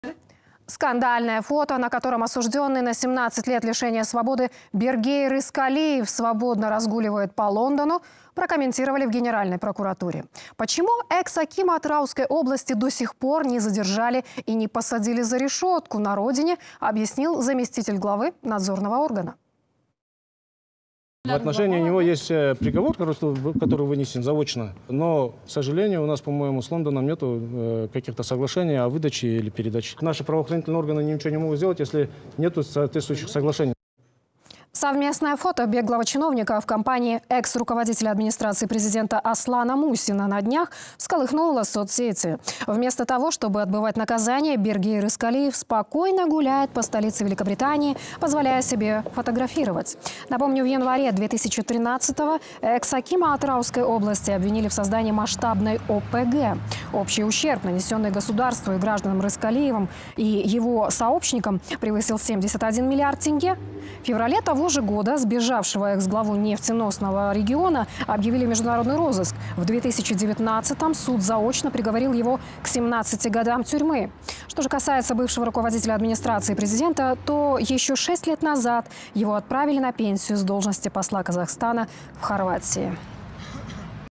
Лондонское фото беглого экс-акима Атырауской области прокомментировали в Генпрокуратуре
Скандальное фото, на котором осужденный на 17 лет лишения свободы Бергей Рыскалиев свободно разгуливает по Лондону, прокомментировали в Генеральной прокуратуре. Почему экс-акима Атырауской области до сих пор не задержали и не посадили за решетку на родине, объяснил заместитель главы надзорного органа Асет Чиндалиев.